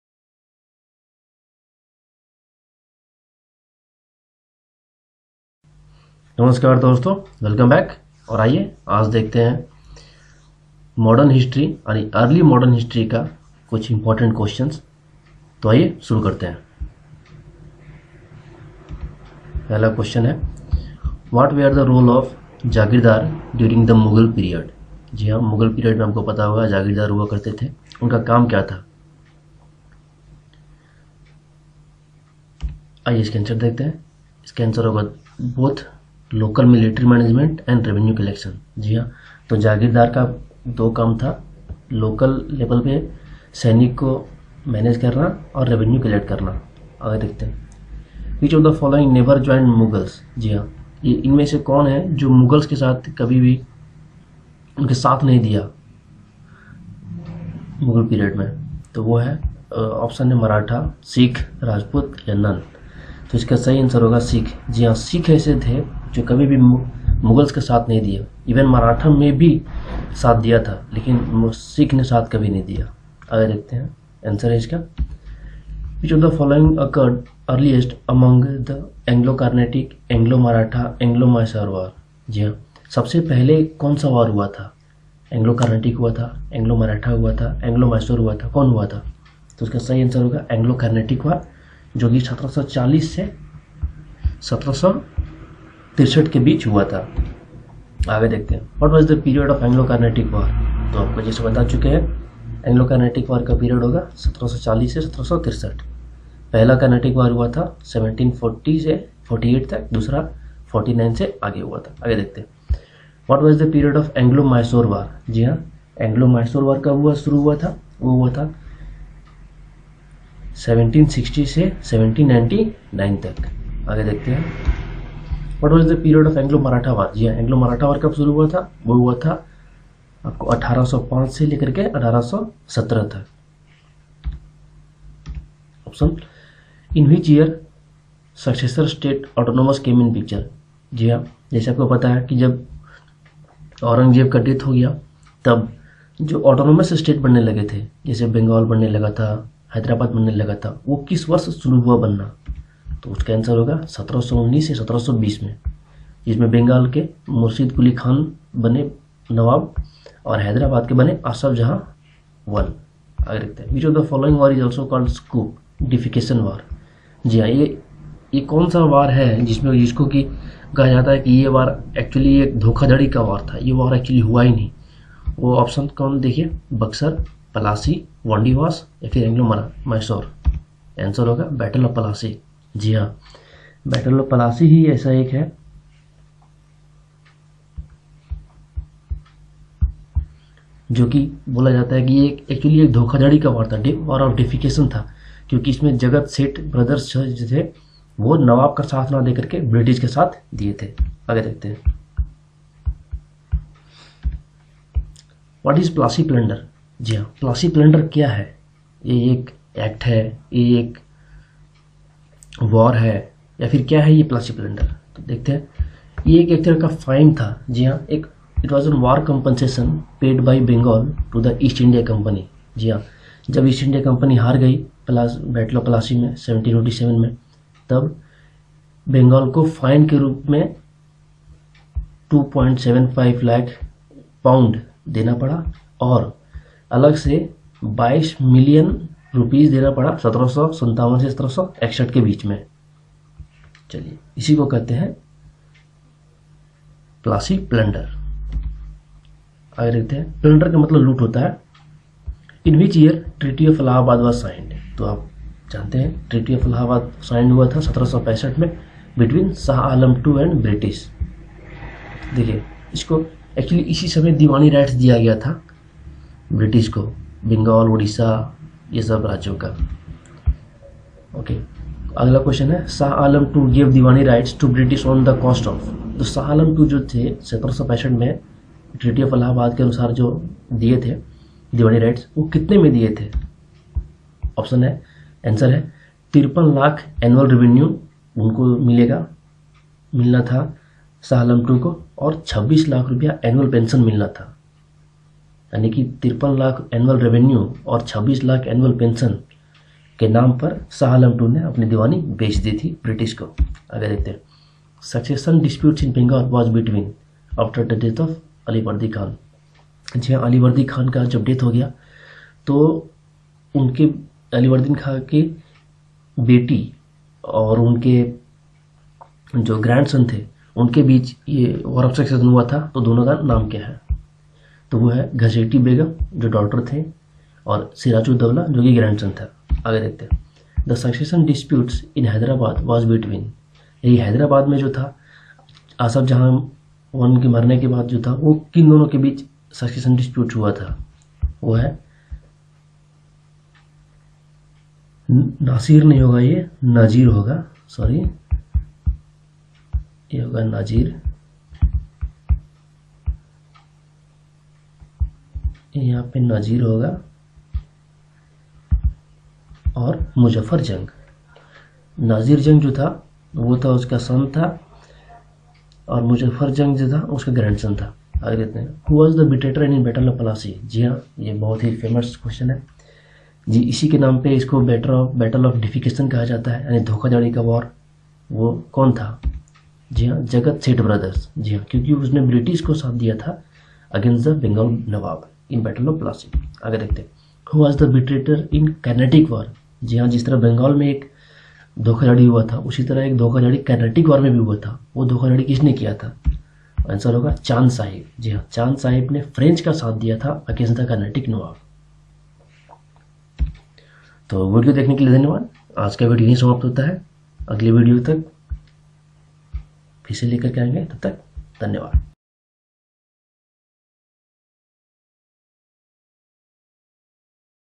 EARLY MODERN HISTORY QUESTIONS FOR PCS-SSC & OTHER GOVERNMENT EXAMS(SSC;RAILWAY Etc)
EXAM ORIENTED EARLY MODERN HISTORY(FALL OF MUGHALS TO RISE OF BRITISH) QUESTIONS FOR PCS-SSC & OTHER GOVERNMENT EXAMS!
TECHNOLOGY RELATED QUESTIONS! IMPORTANT FOR STATE PCS(JPSC-BPSC etc) & other government exams